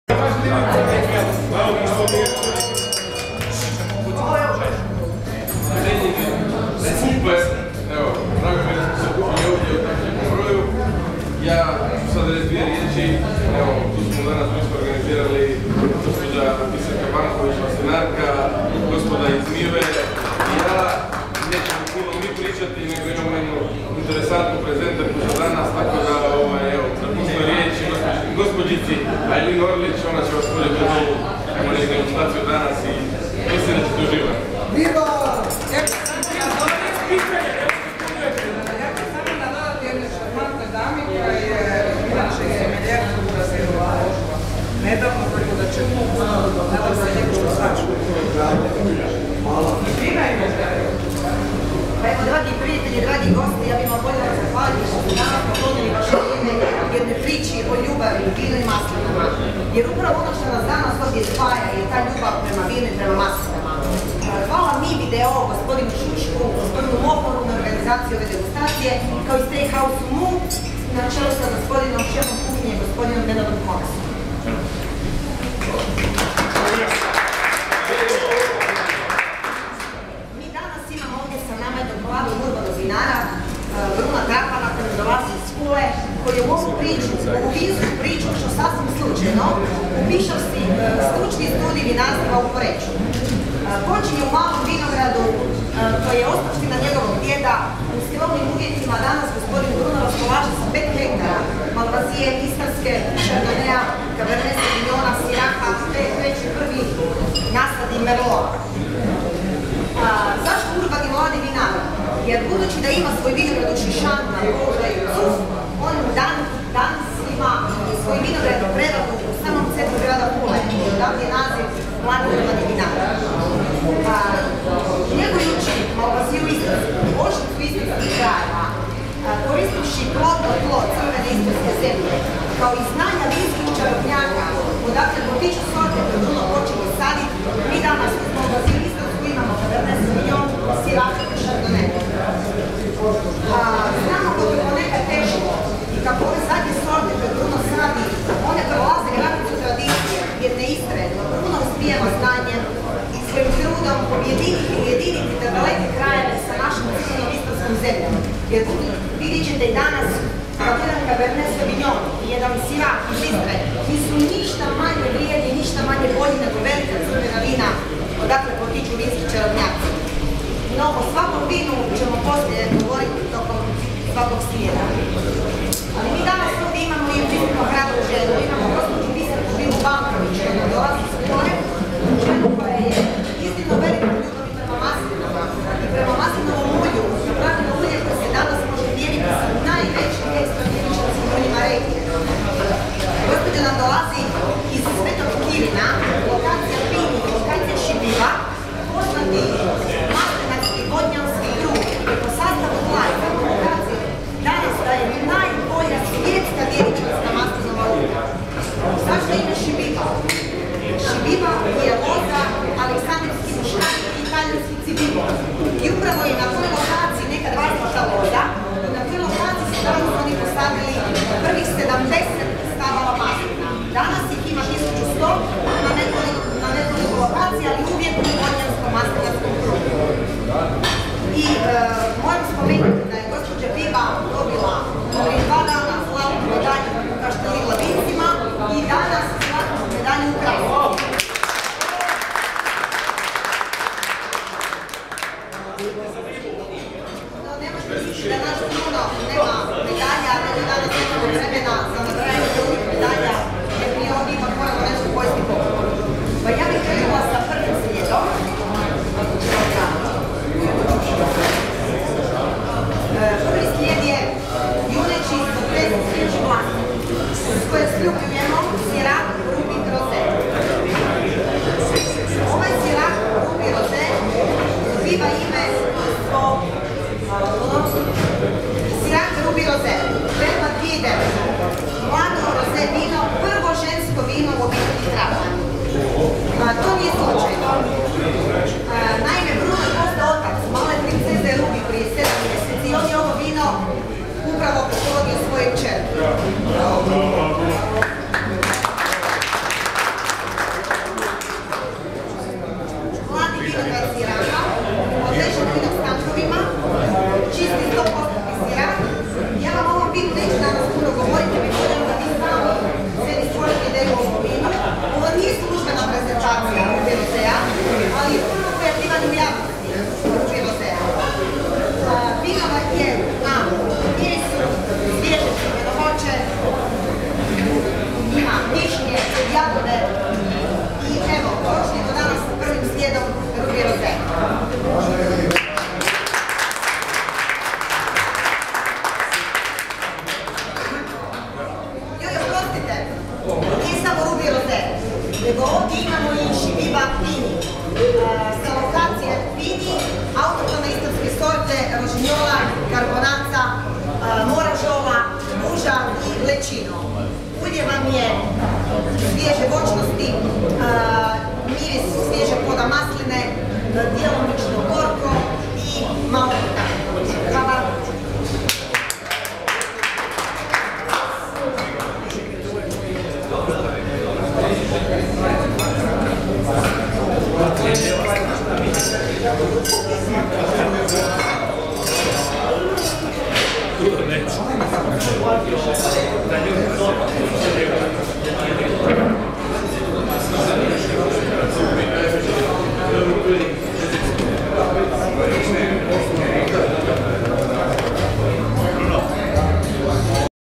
Trapan i Šibiba promocija u Mu
Promocija Trapan vina i Šibiba ulja u steakhouse Mu, Zagreb. Moderator Aileen Orlić.